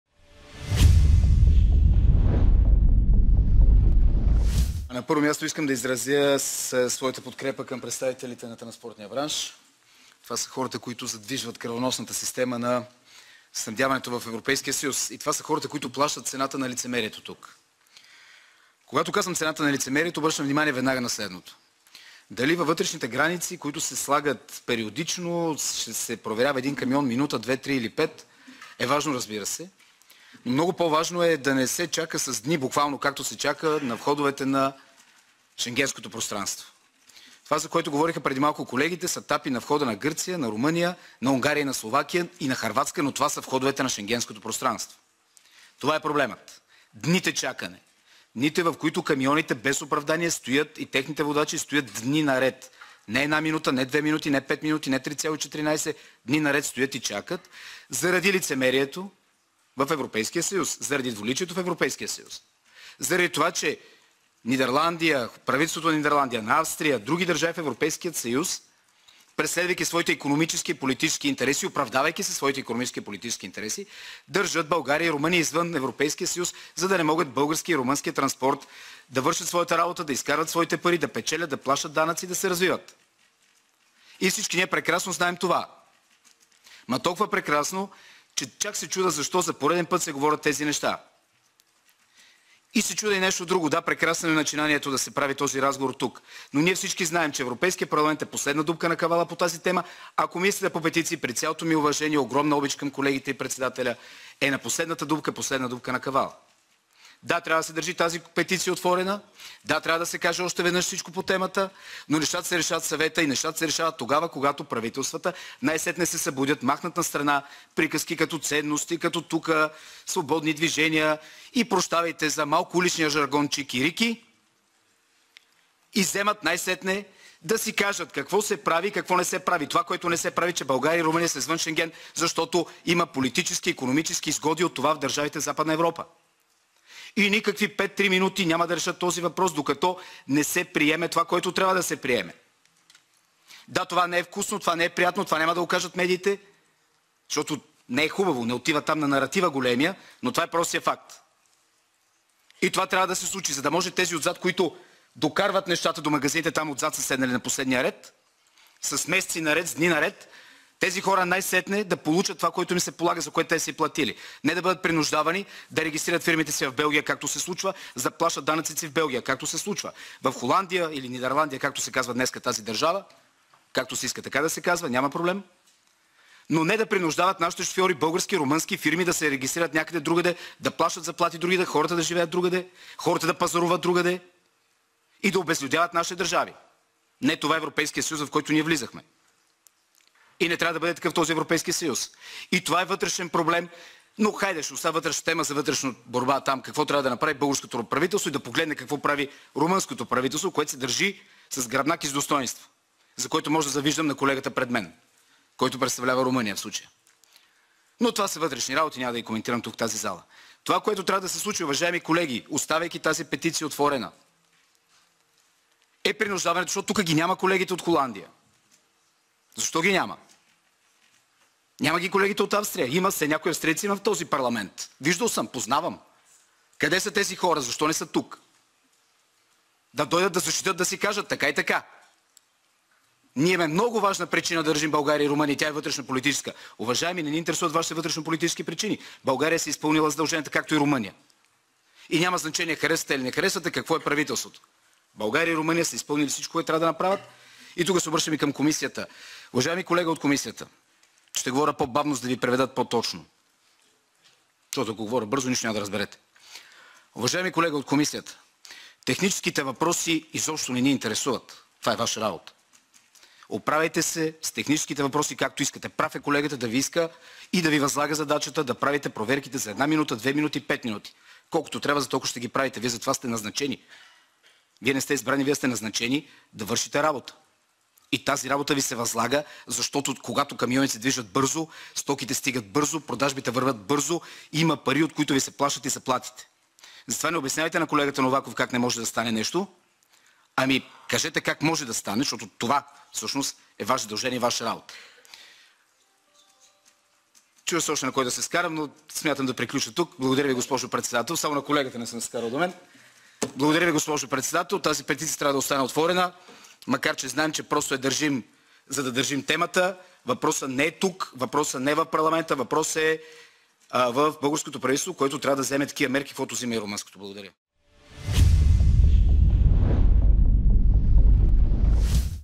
Ангел Джамбазки: Превозвачите плащат цената на брюкселското лицемерие
Ангел Джамбазки подкрепи българските представители на транспортния бранш в Комисията по петиции в Европейския парламент.
Превозвачите задвижват кръвоносната система на снабдяването в ЕС и плащат висока цена, заради брюкселското лицемерие, заяви евродепутатът от ВМРО. Вижте какво каза в своето изказване Ангел Джамбазки.